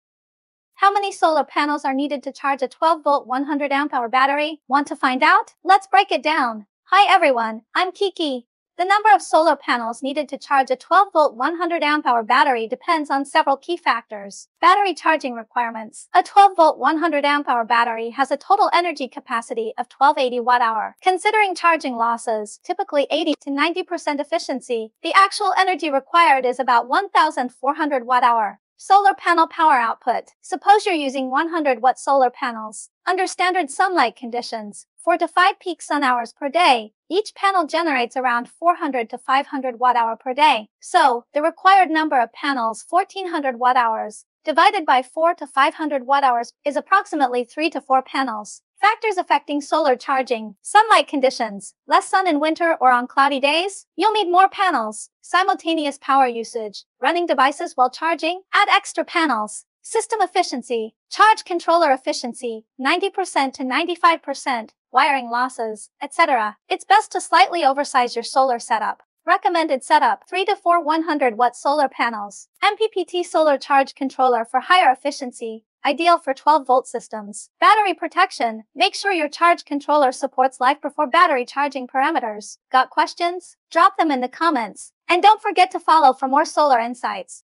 How Many Solar Panels Are Needed to Charge a 12volt 100amp-hour Battery?
Wondering how many solar panels are required to fully charge a 12V 100Ah battery? In this video, we break down the calculations step by step, considering factors like sunlight hours, panel wattage, and charging efficiency. Whether you're setting up a solar system for an RV, boat, or off-grid use, this guide will help you choose the right setup.